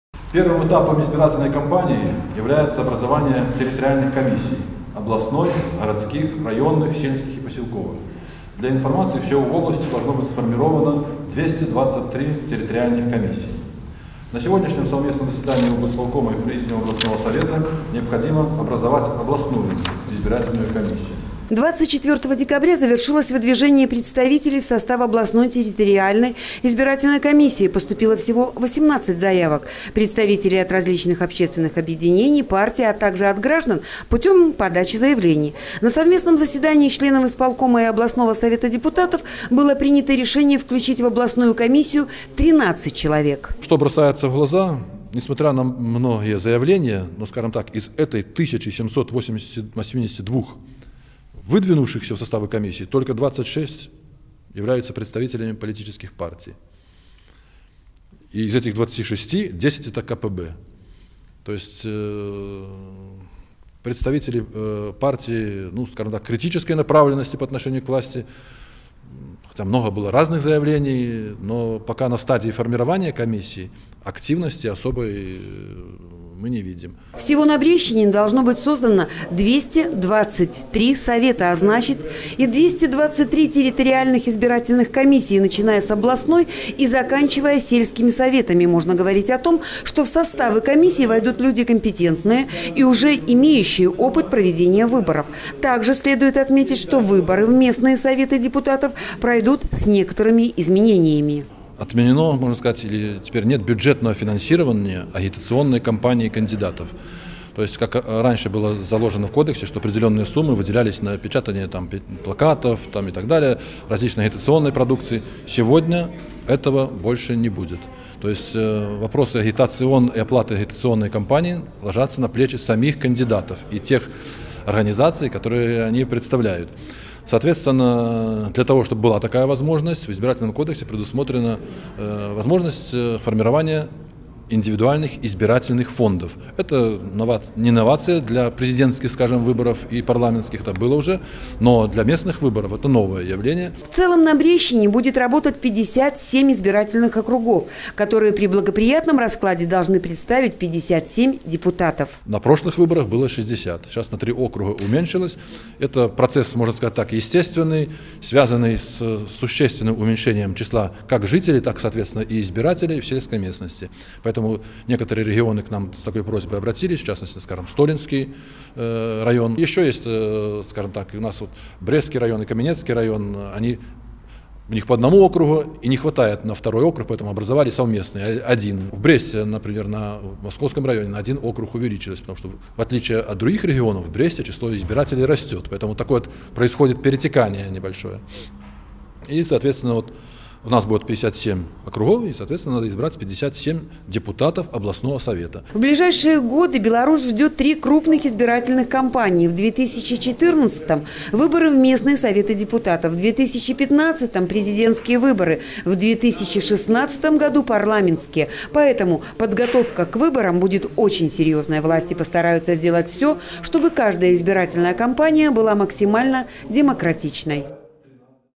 2013-12-26 г. Брест Телекомпания "Буг-ТВ". Заседание облисполкома и Совета депутатов:
Журналист Л. Осмолович. Оператор В. Холявко
Заседание  облисполкома и Совета депутатов: создание территориальной областной комиссии по выборам в местные советы.
Выборы в местные Советы депутатов пройдут в Беларуси  23 марта 2014 года.  Глава государства одобрил дату проведения выборов, отметив, что "хотелось бы, чтобы эти выборы стали демонстрацией истинной демократии».
А сегодня  прошло совместное заседание Брестского областного исполнительного комитета и президиума Брестского областного Совета депутатов по вопросу формирования Брестской областной территориальной избирательной комиссии по выборам депутатов местных Советов депутатов двадцать седьмого созыва.